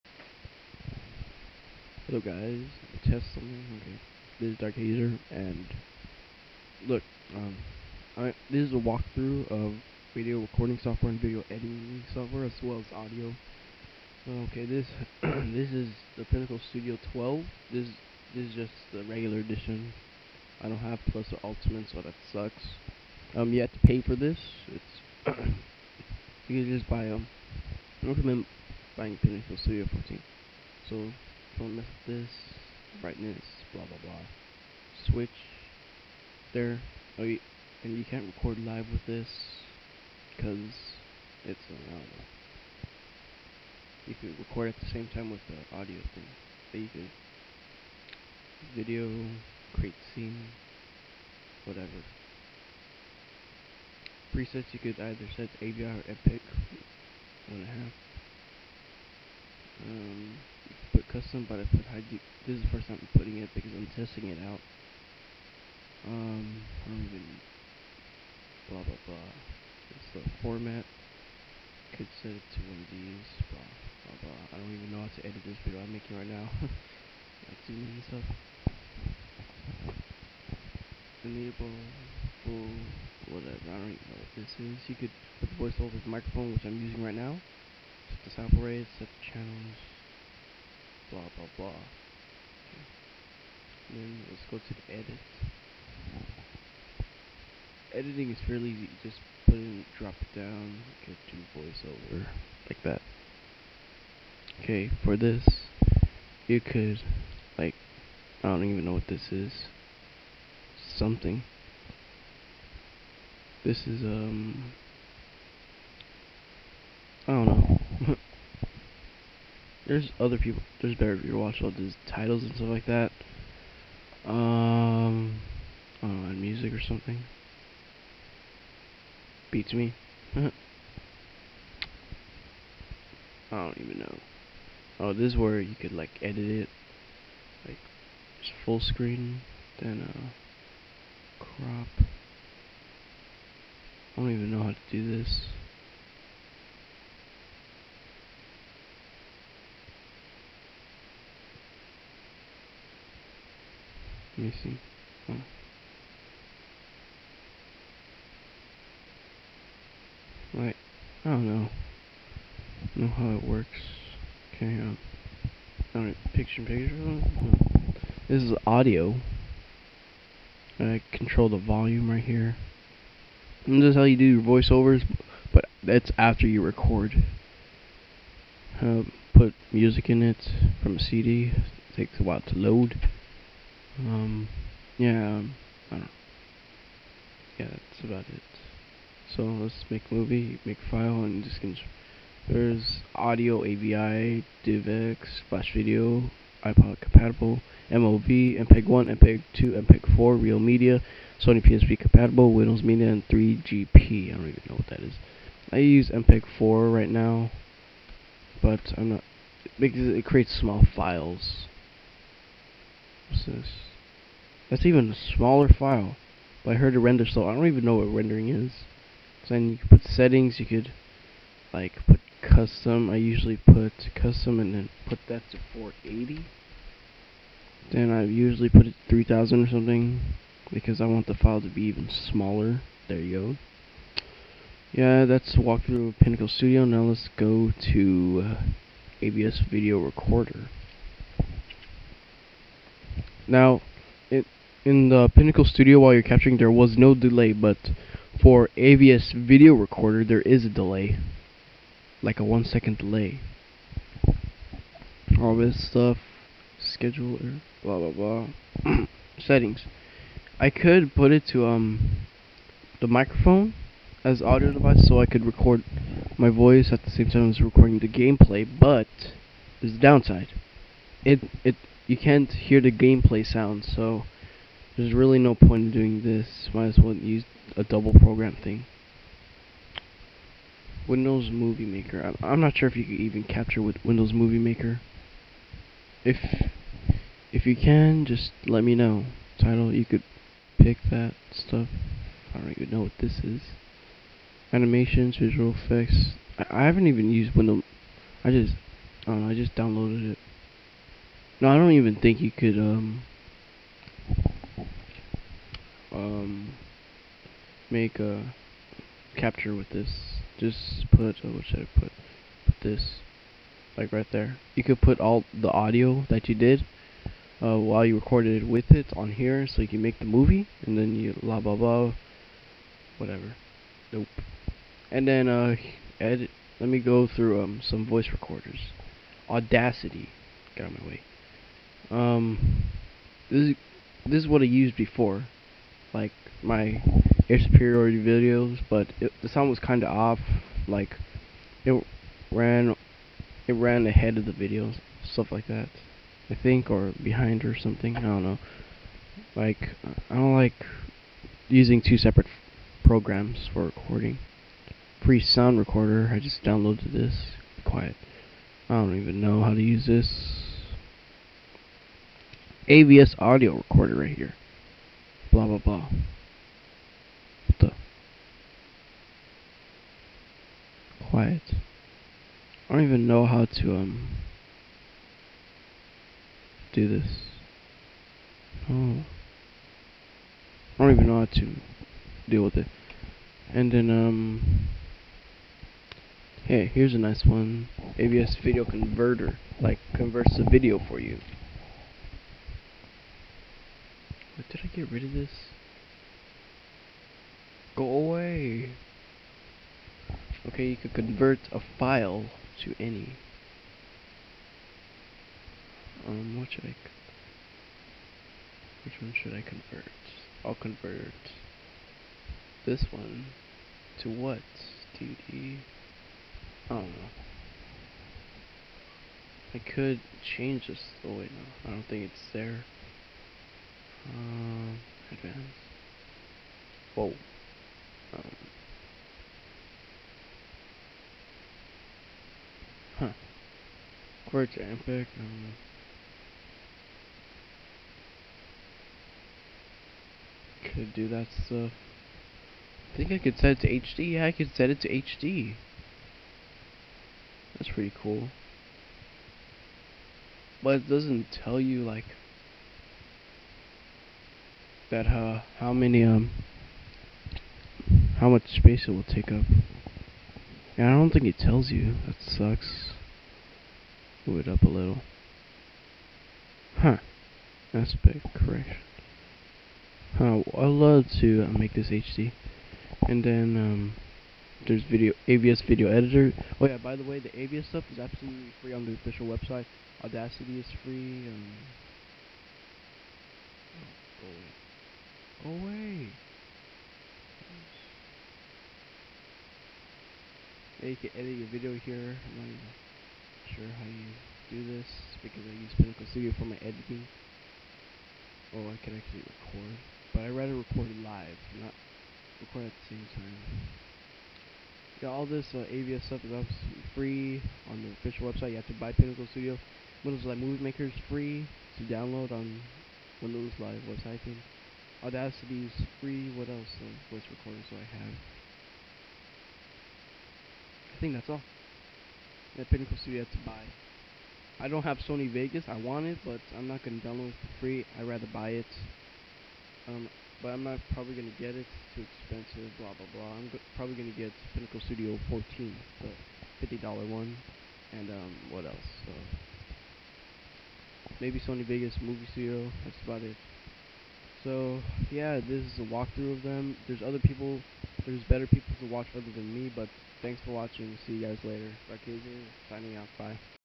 Video Recording and Editing Software Walkthrough
This is a walkthrough of video recording, editing, audio recording, and editing software.